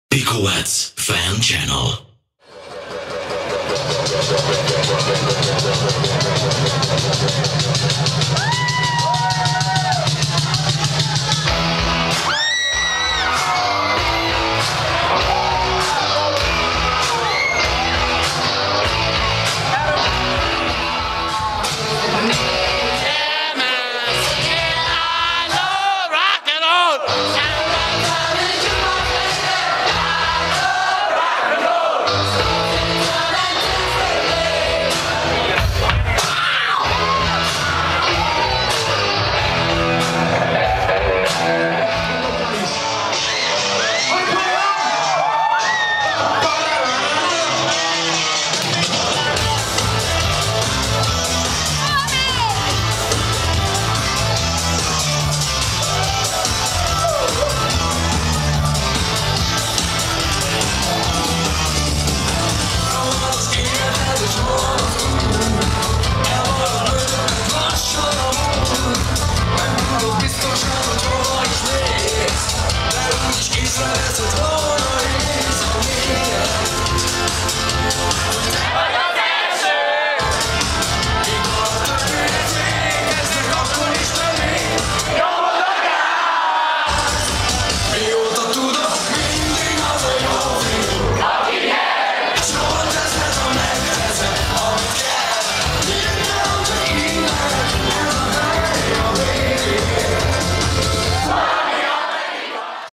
2012.10.18 - Pikowatt's Fan Channel - XII. Kozgaz golyabal, Dj Arpi Varga [Pici] - Kolozsvar
- XII. Közgáz gólyabál, Club Ring - Kolozsvár; Dj Arpi Varga [Pici]